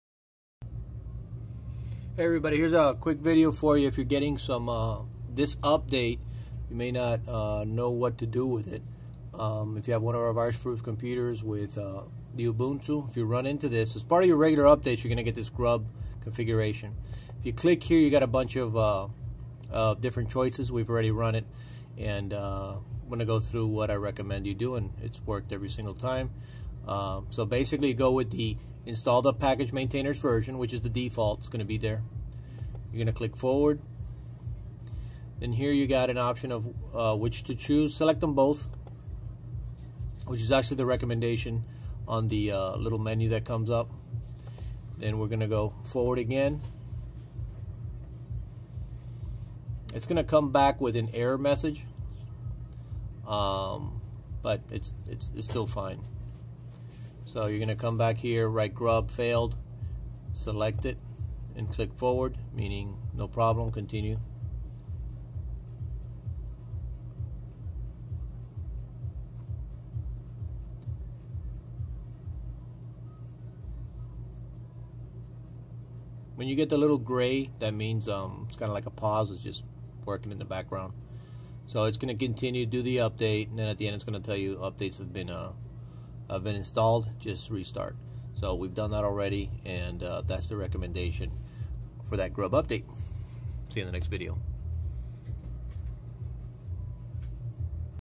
Configuring Grub Update on Ubuntu
- Follow these steps on the recent Grub update and you will be good to go.